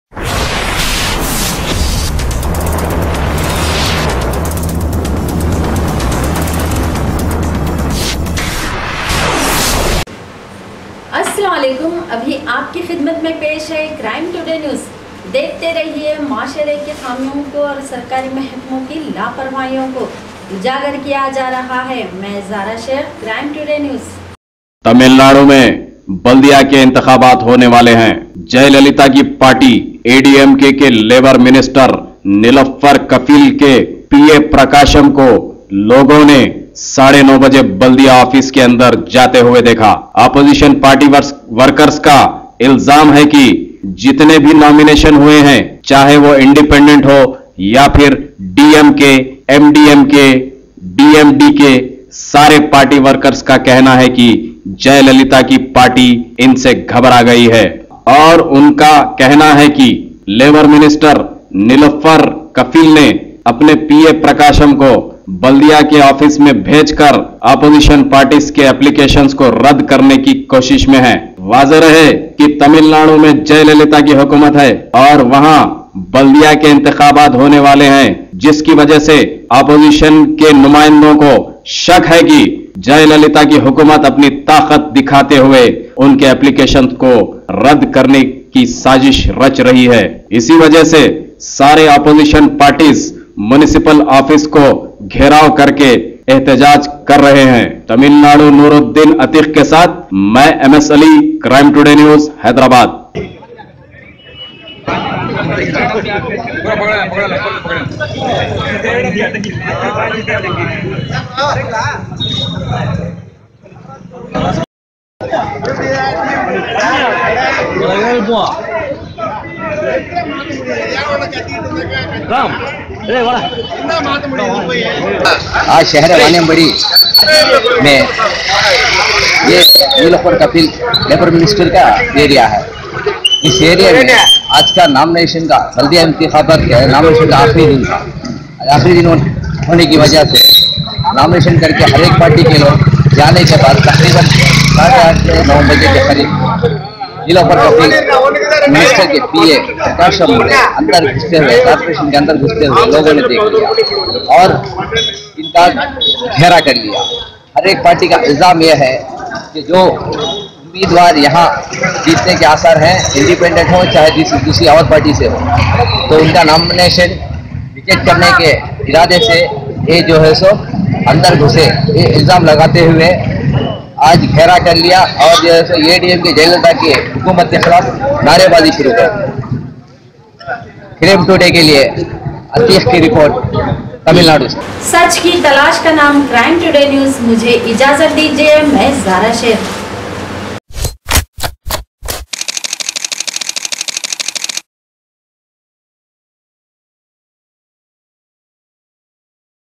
TAMILNADU ALL PARTY WORKERS PROTEST AGAINST JAILALITA
Logon HYDERABAD TAMILNADU ALL PARTY WORKERS PROTEST AGAINST JAILALITA

24/7 Live Online